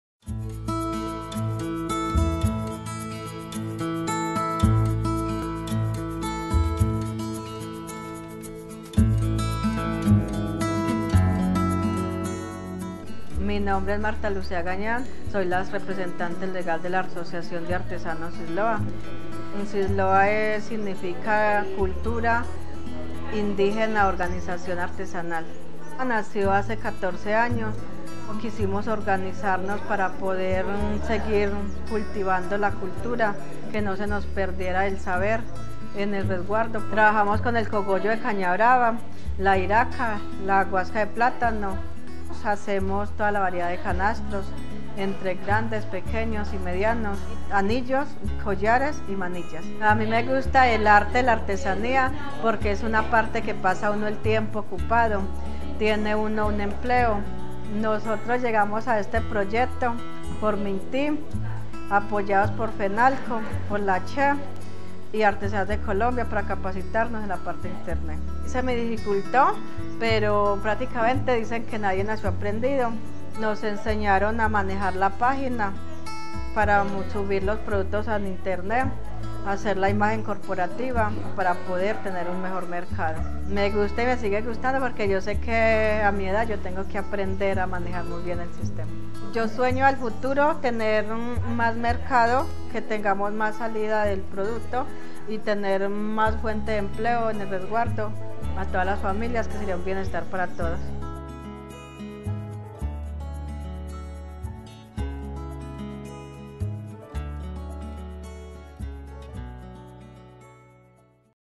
La riqueza artesanal de Colombia traspasa fronteras con las TIC
Empresarios artesanales del resguardo San Lorenzo, impulsan sus negocios con TransformaciónDigital, la presencia de un negocio por internet aumenta sus ventas.